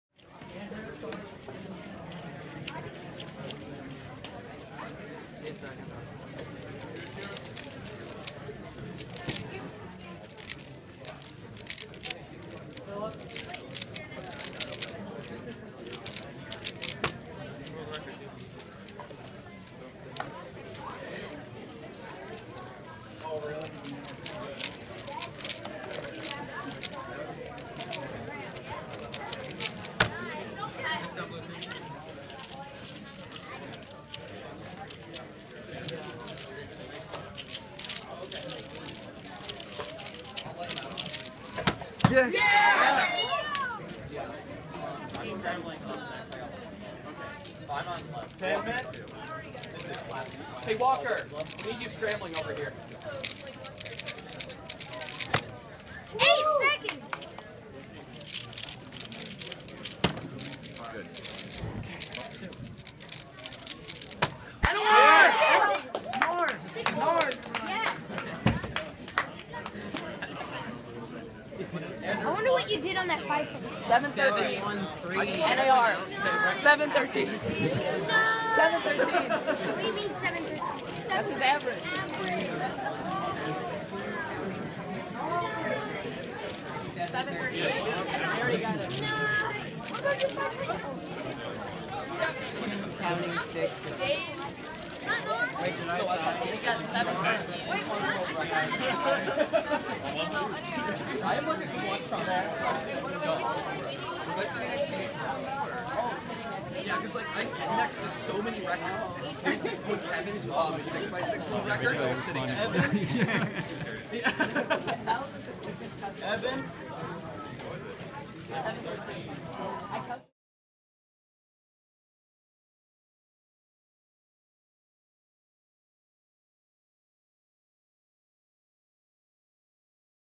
Lucas Etter - 7.13 3x3 Official (Former) North American Record Average!
Great average! Second in the world at the time of the average. Done at Michigan 2015 in Ann Arbor, Michigan.                                                    Cube: Aolong v2